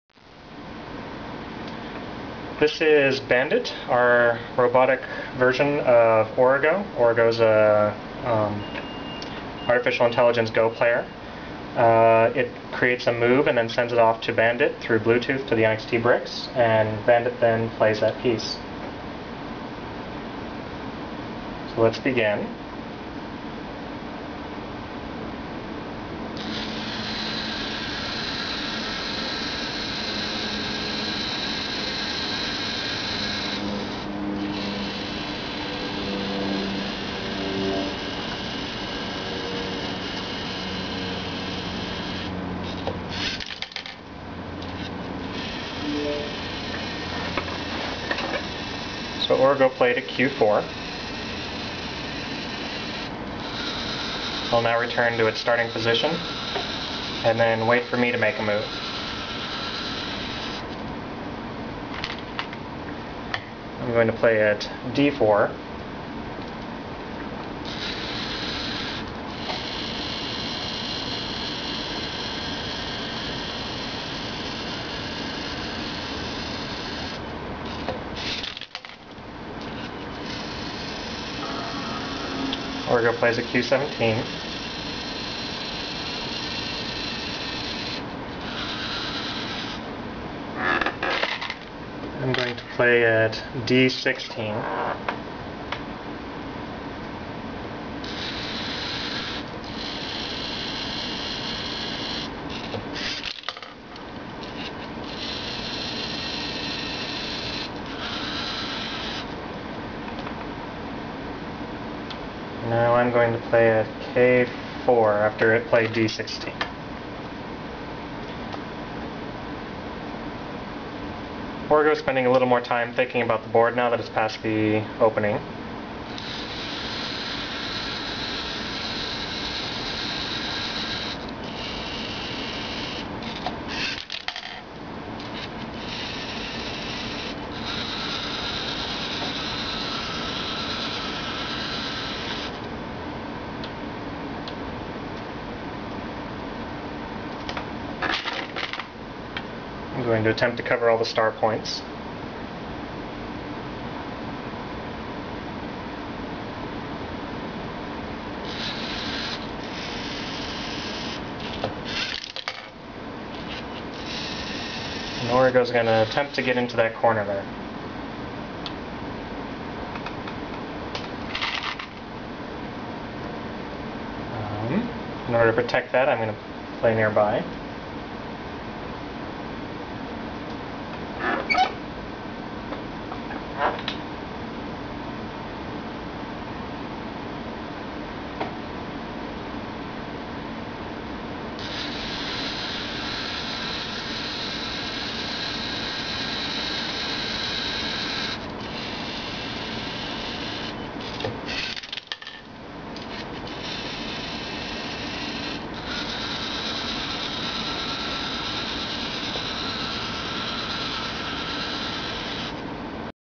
Go-playing robot
Bandit the robot plays the game of Go. Bandit was developed as part of the Orego project: